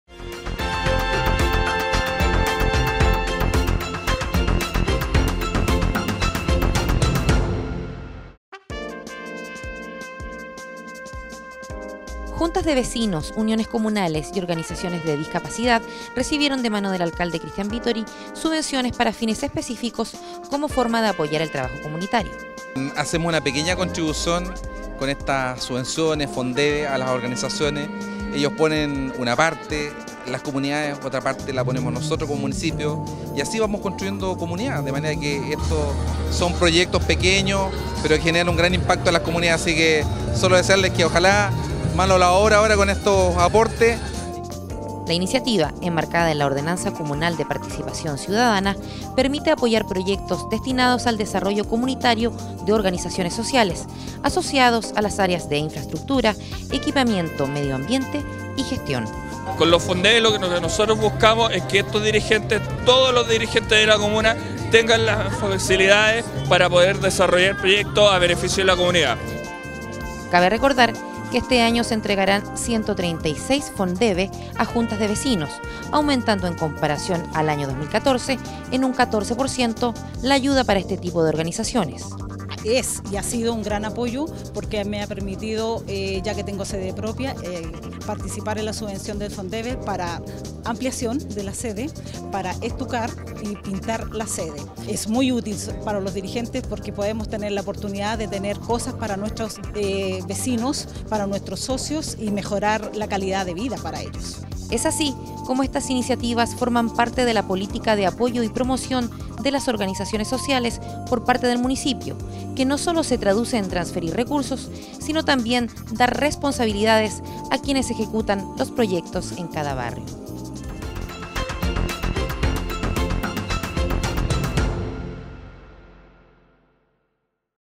ASIGNAN NUEVOS FONDEVE A ORGANIZACIONES SOCIALES
En la ocasión recibieron de mano del alcalde Christian Vittori una ayuda monetaria como forma de apoyar el trabajo comunitario.